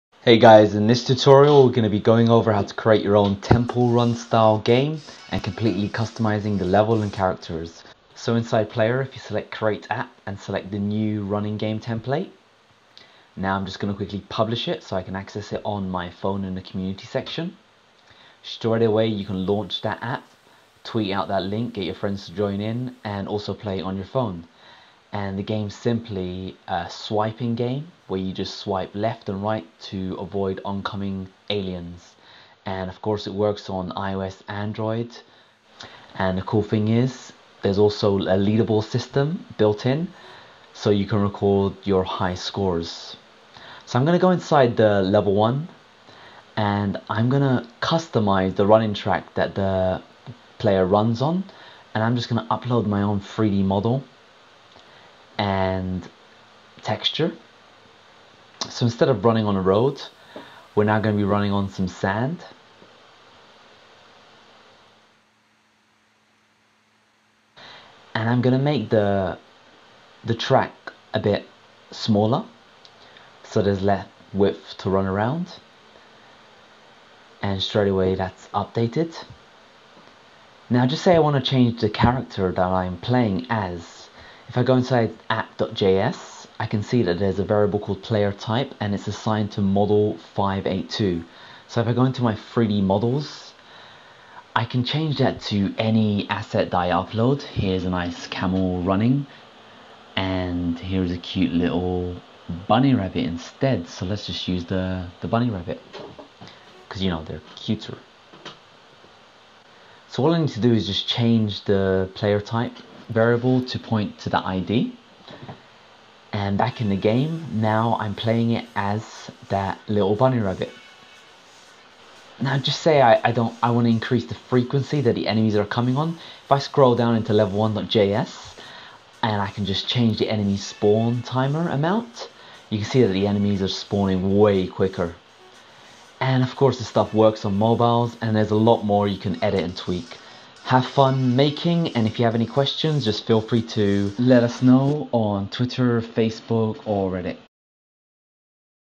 Create your own Temple Run style game
ts too intresting
Play and enjoy it
The Make Your Own Game series teaches you how to create a hit mobile video game using the Buildbox game maker. In this video, Buildbox founder, Trey Smith, discusses how the series will work and gives you a glimpse of what you will create.